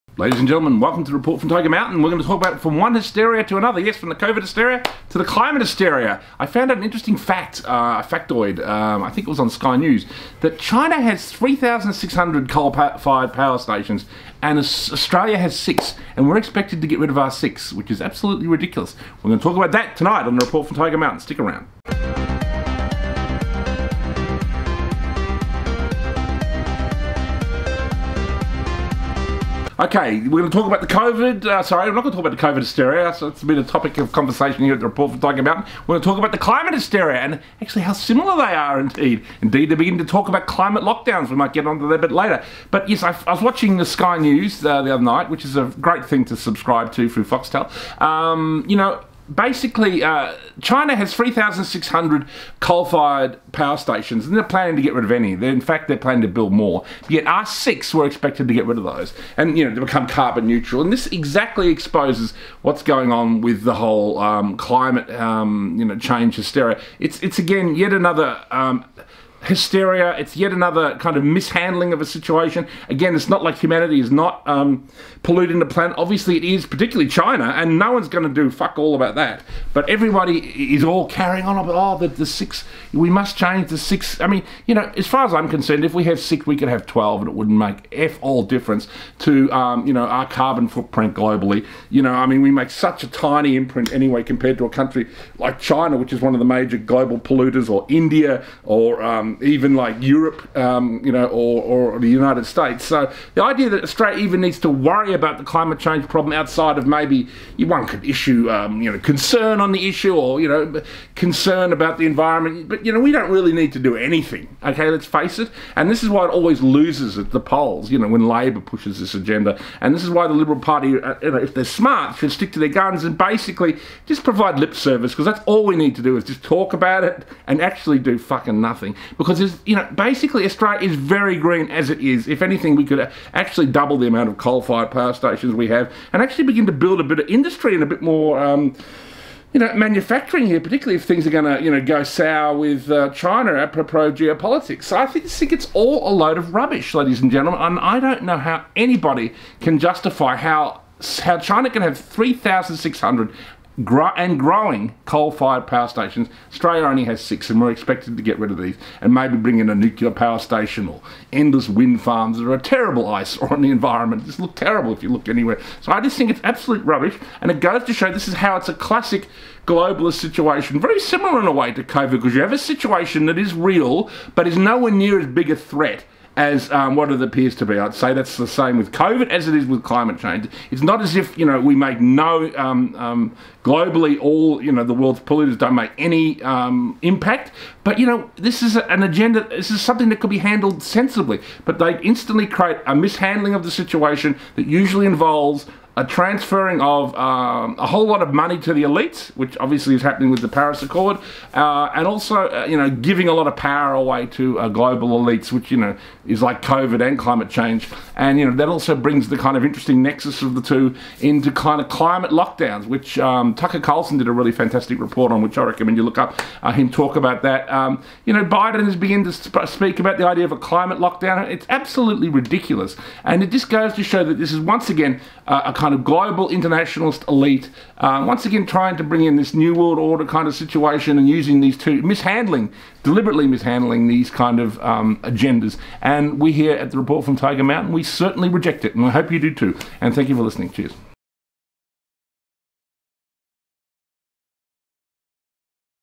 The Return of Climate Hysteria | Report from Tiger Mountain
In this Report, Richard comments on the return of climate hysteria focusing on the fact that Australia and the West are expected to close down their coal fire-powered station while China builds more.